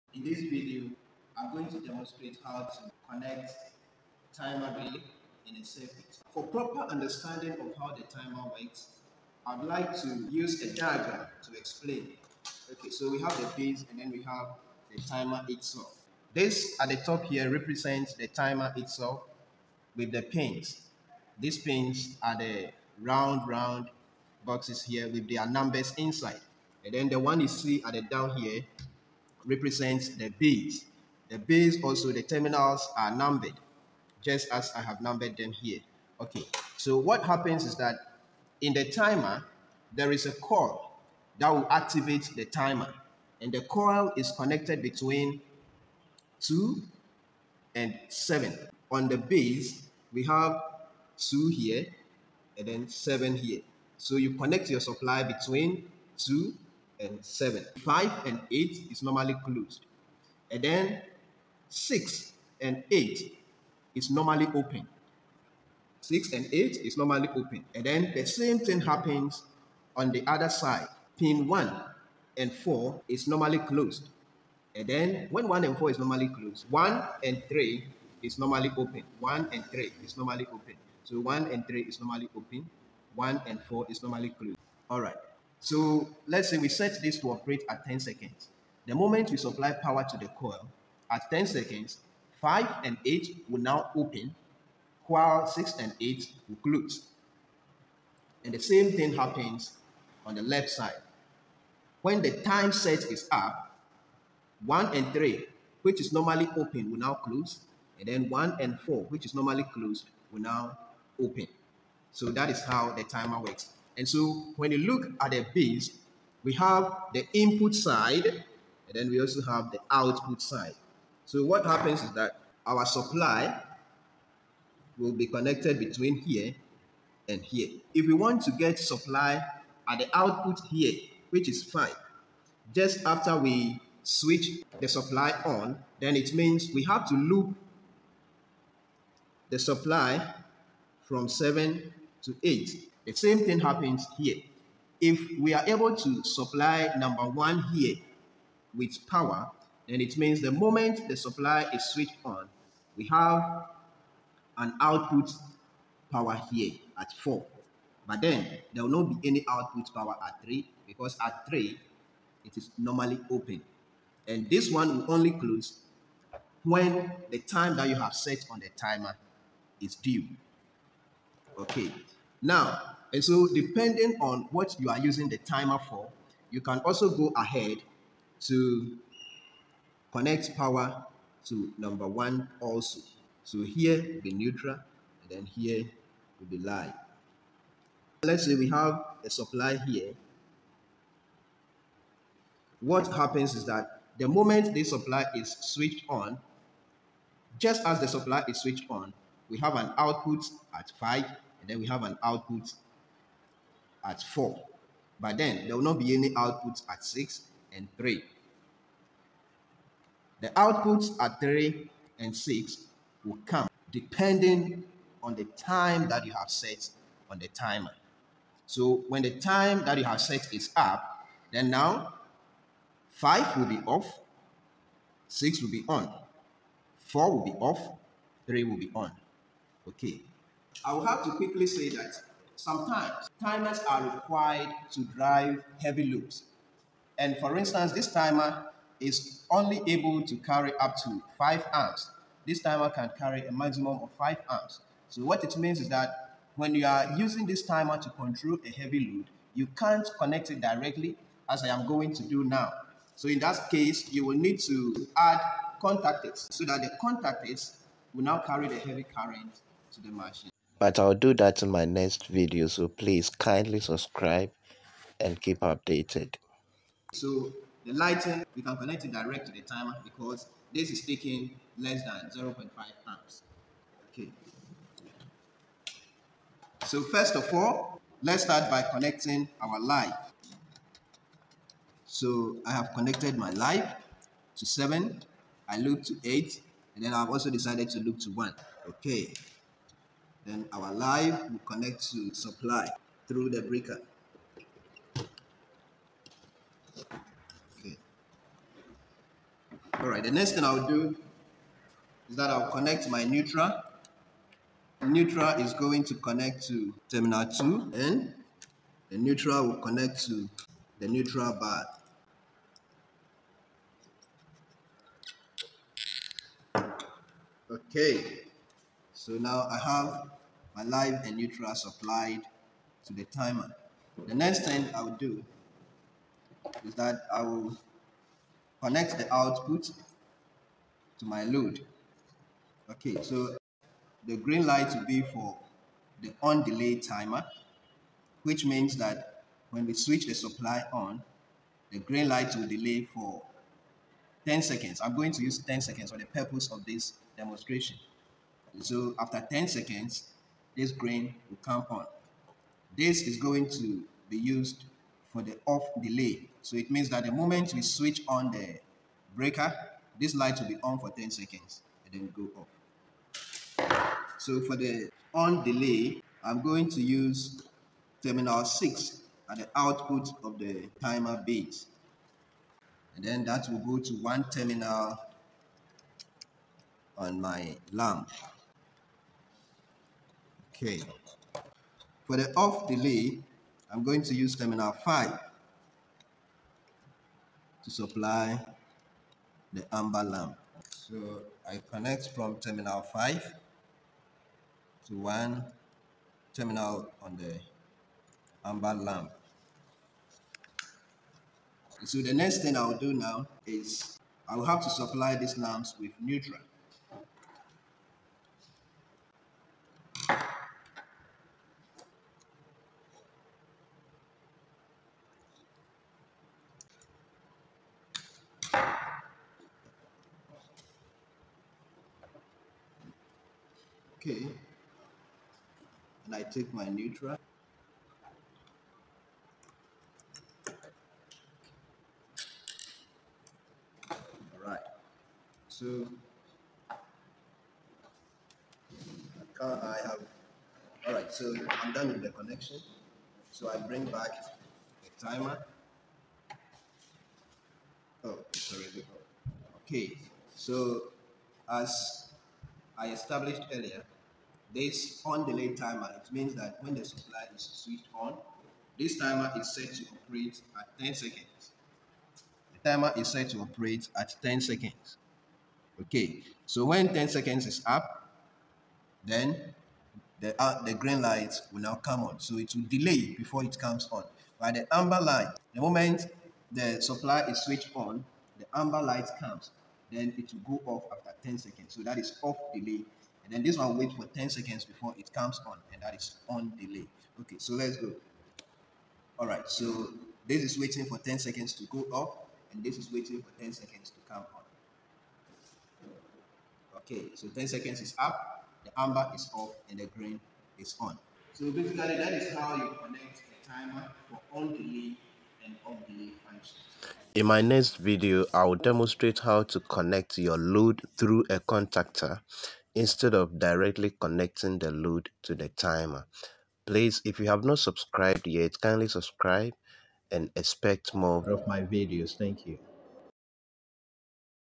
HOW TO WIRE AN 8PIN TIMER FOR |"ON DELAY" AND "OFF DELAY" TIMING
This video is a demonstration of how to wire an 8pin timer relay, for both "on delay and "off delay functions.
in the video, two indicating lamps (green and amber) were used to represent the loads for the "on delay" and "off delay" functions respectively. In the video, the lamps are connected directly to the timer. This is because the lamps used need very little amount of current to operate. But this will not be the case, when the load needs a substantial amount of current to operate. In that case, a contactor must be involved.
To know how to connect the timer to the load through a contactor, kindly